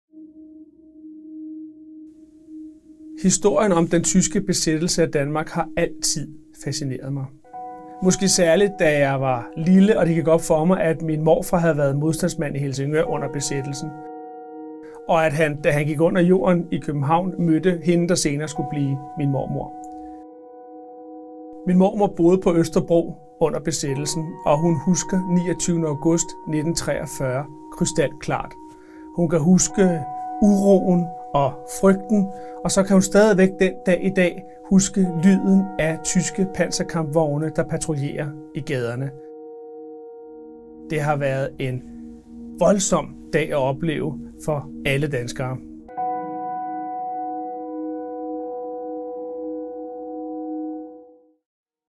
Simon Kratholm Ankjærgaard fortæller om sin bog Augustoprøret 1943 | Lindhardt og Ringhof
Den 29. august 1943 brød den danske samarbejdspolitik med tyskerne endeligt sammen, og Danmark tog fast plads i den allierede lejr.

Interneringen af den danske hær og sænkningen af den danske flåde 29. august har siden stået som ikoniske billeder på samarbejdspolitikkens sammenbrud, men i virkeligheden begyndte kollapset en måned før – og bredte sig som en bølge af folkelige protester fra Odense til Esbjerg, Ålborg, Nyborg og Korsør, før det endte i hovedstaden.

I AUGUST 1943 fortæller Simon Ankjærgaard den dramatiske og ofte glemte historie om danskernes trodsighed hen over landet i form af både sabotage og store demonstrationer. Det sker med udgangspunkt i nogle af hovedpersonerne i de forskellige byer.

Bogen er blevet til gennem research i store og små arkiver og udkommer rigt illustreret i stort format.

Lindhardt og Ringhof